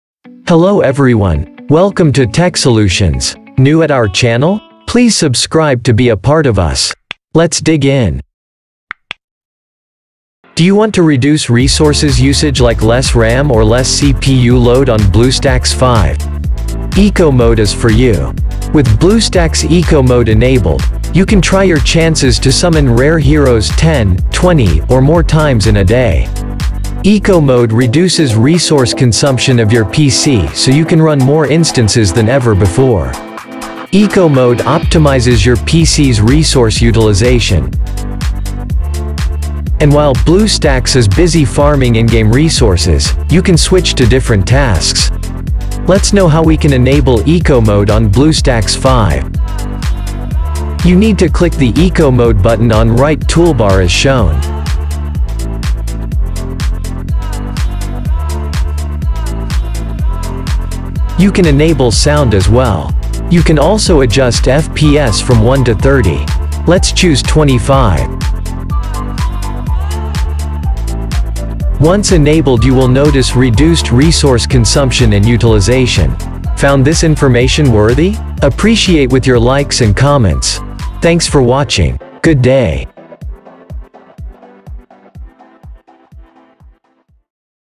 How to Turn on Eco Mode on BlueStacks 5 | Enable BlueStacks 5 Eco Mode | BlueStacks 5 Settings
How to Turn on Eco Mode on BlueStacks 5 | Enable BlueStacks 5 Eco Mode | BlueStacks 5 Settings With BlueStacks’ Eco Mode enabled, you can try your chances to summon rare heroes 10, 20, or more times in a day. And while you’re at it, the Eco Mode reduces resource consumption of your PC so you can run more instances than ever before. Eco Mode optimizes your PC’s resource utilization. Once enabled, it leads up to an 87% drop in CPU usage and up to 97% drop in GPU usage. And while BlueStacks is busy farming in-game resources, you can switch to different tasks. Watch a video, read about the best strategy to level up in a game, and more. The all-new Eco Mode gives you the freedom to optimize resource consumption at individual BlueStacks instance level through Multi-instance Manager. It also lets you fine-tune changes like reducing the FPS to as low as 1 on any instance and enable/disable sound. The lower you keep the FPS, the lower would be the system resource consumption which would help you farm more.